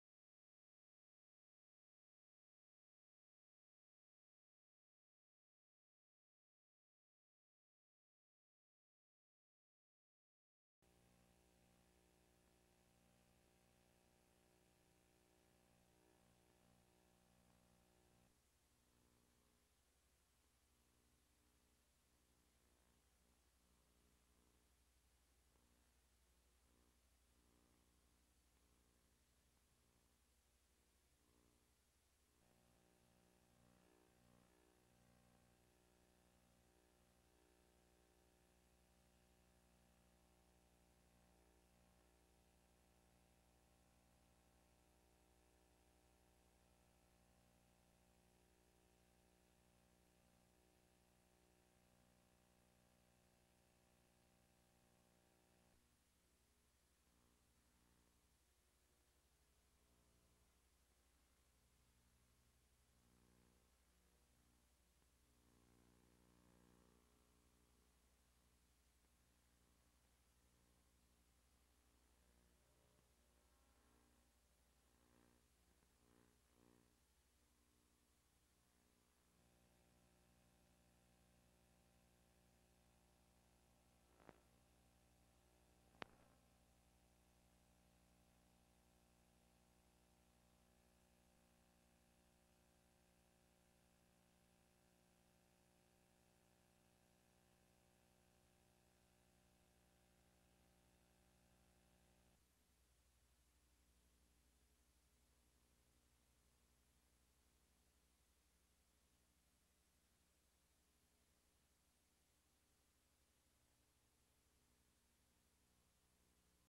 AutoCopter with Infrared Video
The AutoCopter A model hovers while tracking people on the ground.  The video is switched back and forth between visible spectrum and infrared video.